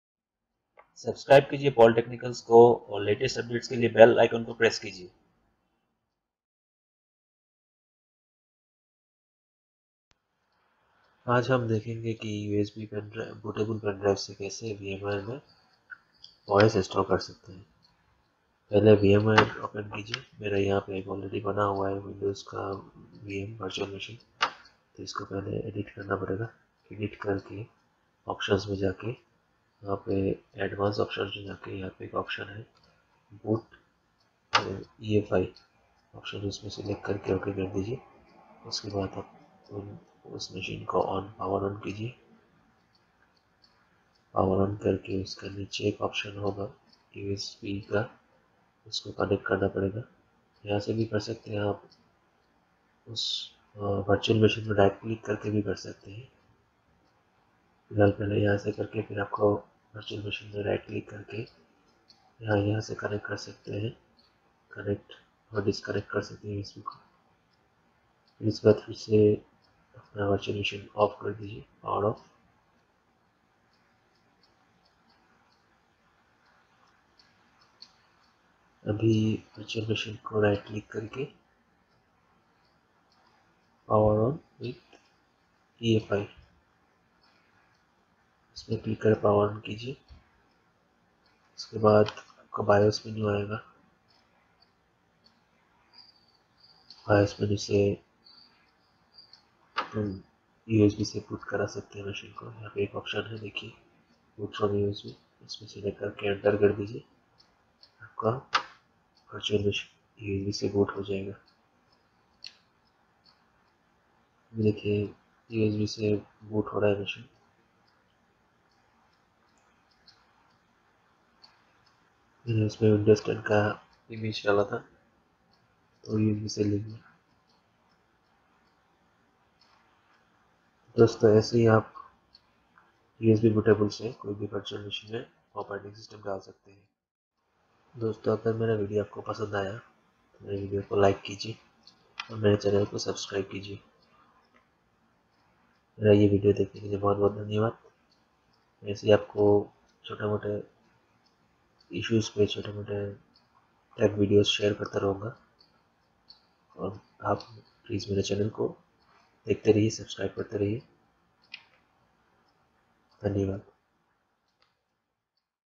How to use Bootable USB in VMware - In Hindi
In This Video You find how to install Operating System in a Virtual Machine (Vmware) using Bootable USB in Hindi
इस वीडियो में आप हिंदी में बूट करने योग्य यूएसबी का उपयोग करते हुए वर्चुअल मशीन (वीएमवेयर) में ऑपरेटिंग सिस्टम को कैसे स्थापित करें